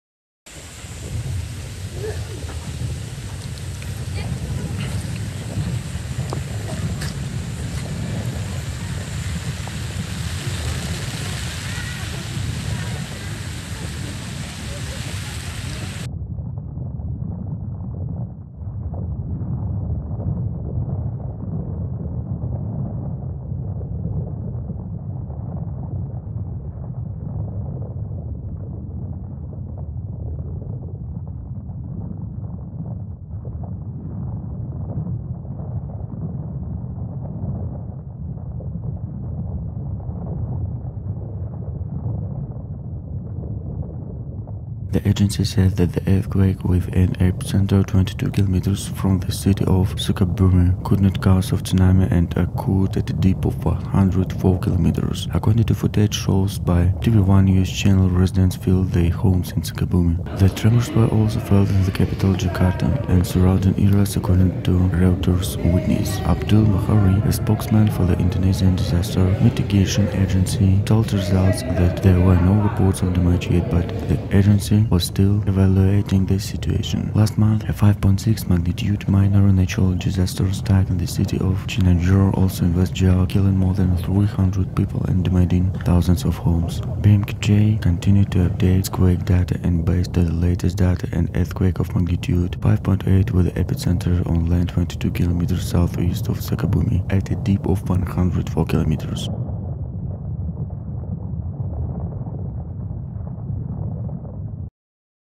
A 5.8-magnitude earthquake rocked Sukabumi in West Java! Indonesia is in total apocalypse!
The agency said that the earthquake with an epicenter 22 km from the city of Sukabumi could not cause a tsunami and occurred at a depth of 104 km.
According to footage shown by TVOne news channel, residents fled their homes in Sukabumi.

The tremors were also felt in the capital Jakarta and surrounding areas, according to Reuters witnesses.
Abdul Muhari, a spokesman for the Indonesian Disaster Mitigation Agency (BNPB), told Reuters that there were no reports of damage yet, but the agency was still evaluating the situation.

a 5.6-magnitude minor natural disaster struck the city of Chianjur, also in West Java, killing more than 300 people and damaging thousands of homes.

BMKG continued to update its quake data, and based on the latest data, an earthquake of magnitude 5.8 with an epicenter on land, 22 km southeast of Sukabumi, at a depth of 104 km.